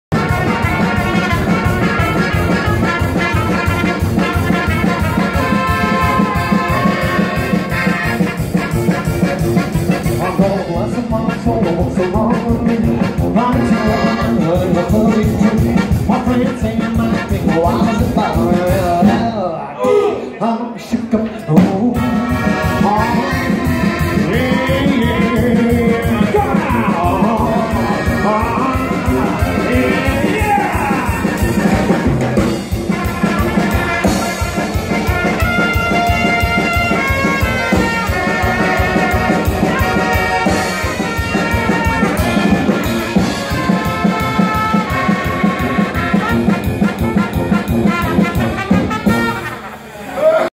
Groot niet te vermijden korte act
Het Groot niet te vermijden is vooral bekend van de vele theatertours. Nu is het gezelschap ook in te zetten bij receptie’s of als bijzonder intermezzo bijvoorbeeld bij congres seminar of feestavond.